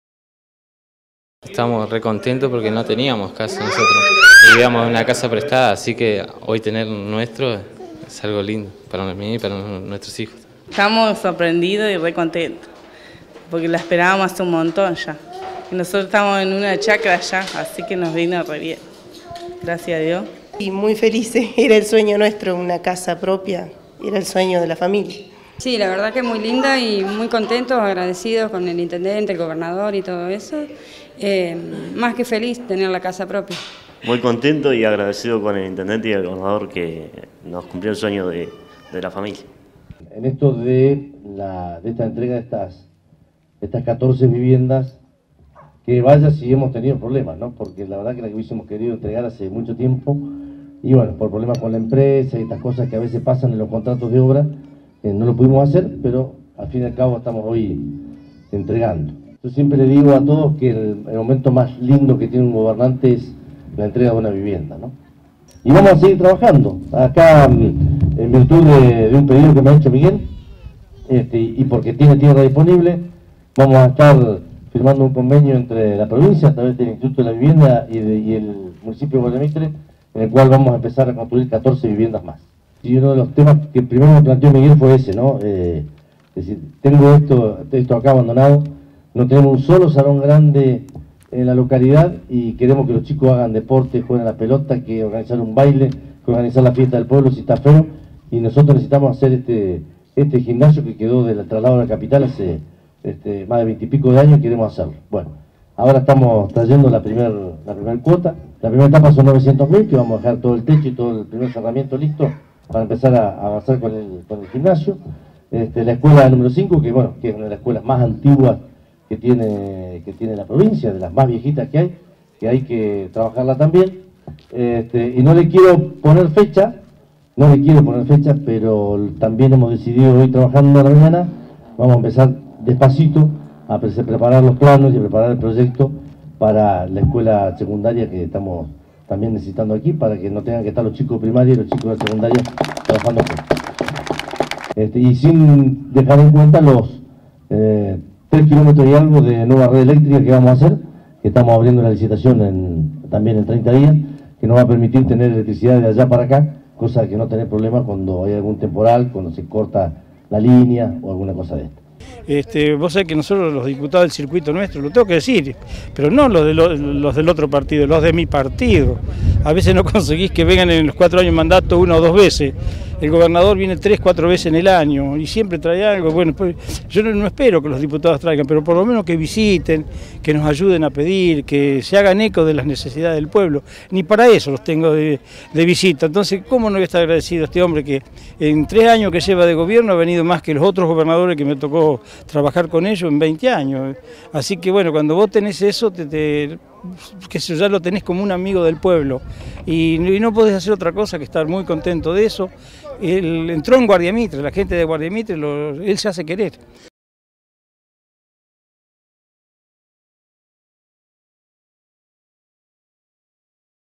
Guardia Mitre- Entrega de casas
El gobernador  Alberto Weretilneck, entregó ayer  en Guardia Mitre 14 viviendas, que demandaron una inversión de $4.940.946.En la oportunidad, anunció la ejecución de otras 14 unidades habitacionales, firmó convenios y entregó aportes que posibilitarán el inicio de obra pública en la localidad.